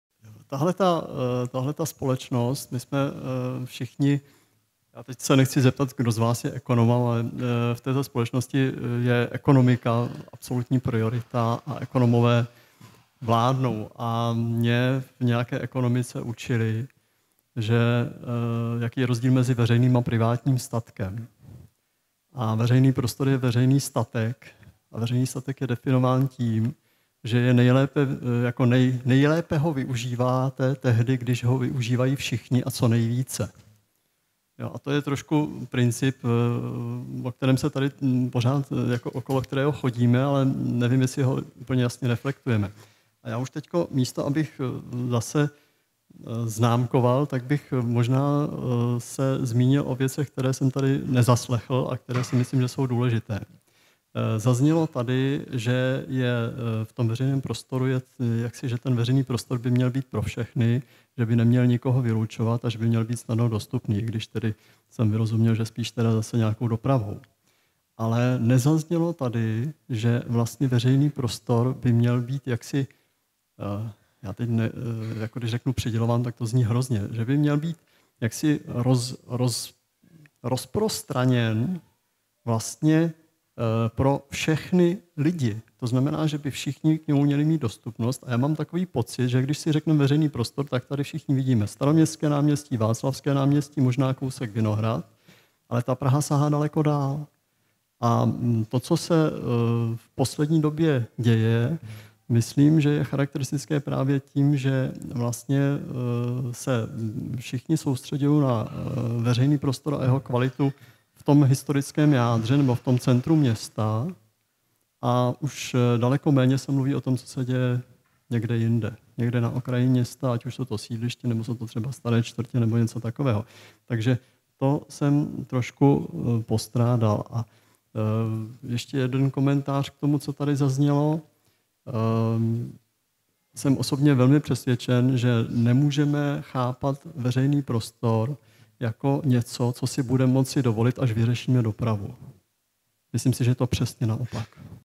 12 Urbanista Karel Maier, komentář k otázce aktivit ve veřejném prostoru.
Komu patří veřejný prostor v Praze? Jaká je jeho cena? Co uděláme se společným prostorem v následujících čtyřech letech? Předvolební debata kandidátů do vedení Prahy. V rámci pouličního festivalu Zažít město jinak v sobotu 25. září, 13 hodin, aula Základní školy v Londýnské ulici č. 34.